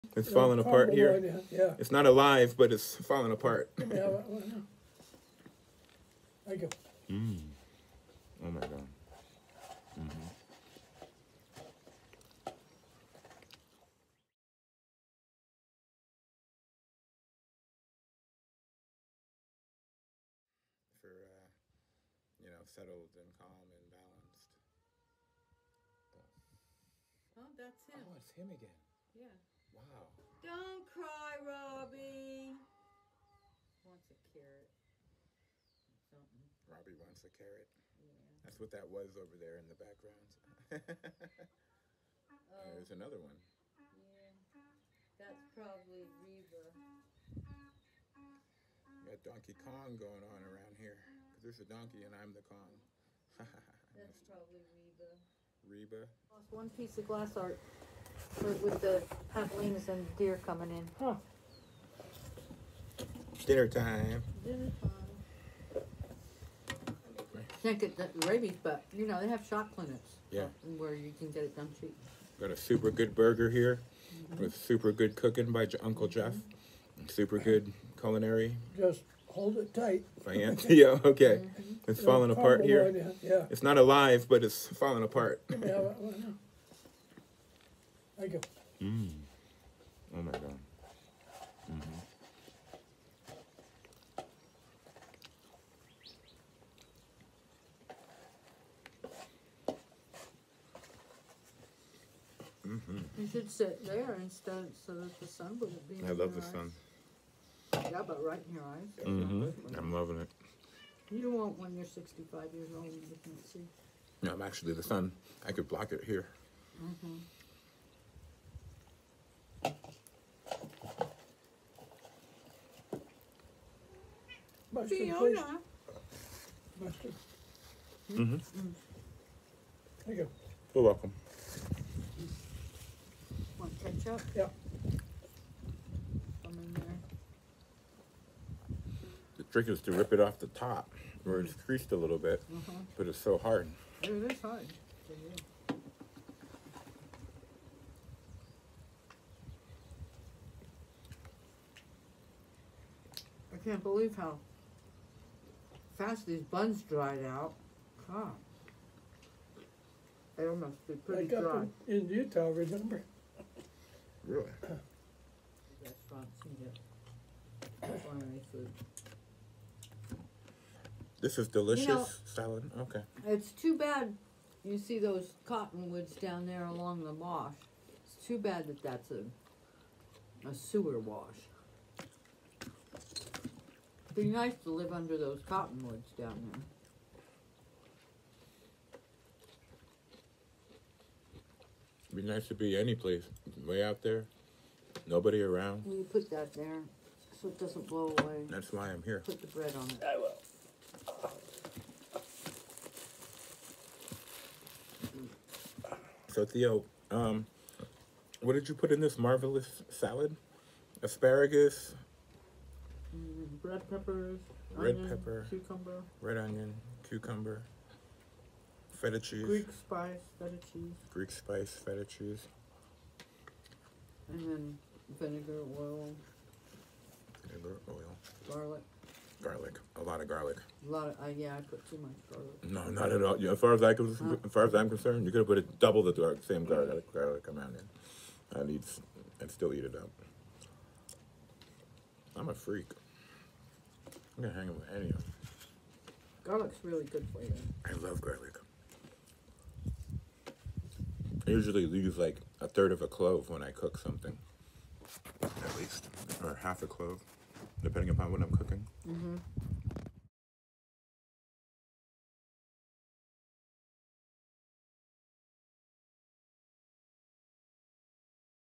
🍔 The Best Hamburger 🍔 at Donkey Ranch Hilarious Background Noises, Arizona 👣🌎☺️🏔🌅🌵
Hilarious Donkey Hollering before Delicious 🤤 Grilled Hamburger 🍔 and Asparagus 🥗 Salad 100’s of feet from the Mexican Boarder during Sunset in the Desert 🌵 🏜 of Arizona.